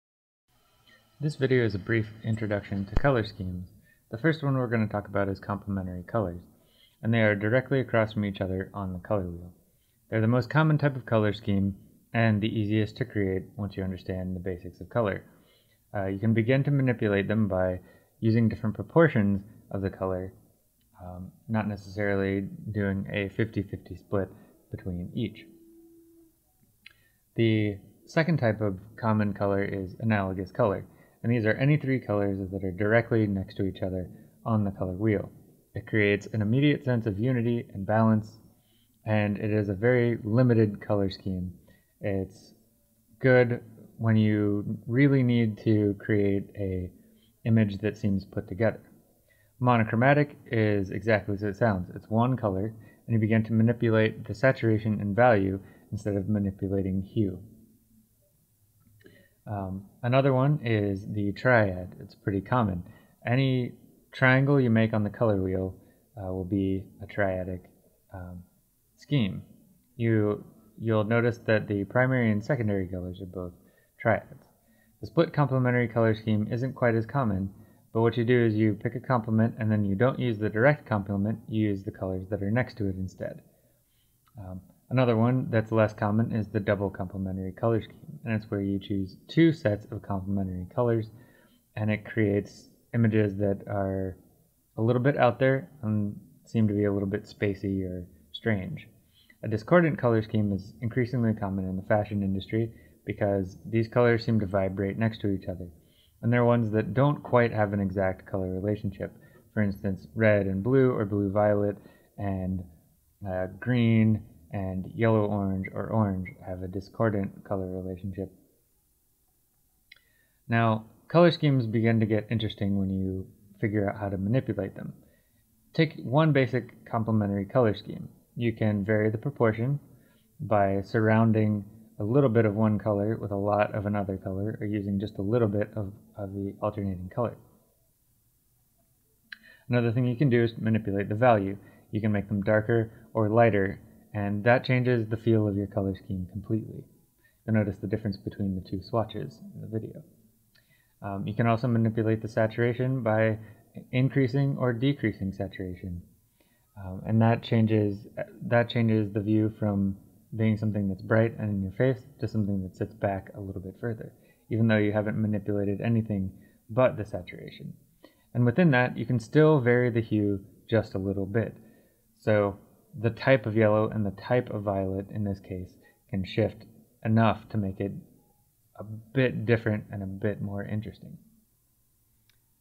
Intro to Color Schemes
This video goes over the basics of color schemes and assumes a little bit of familiarity with the color wheel.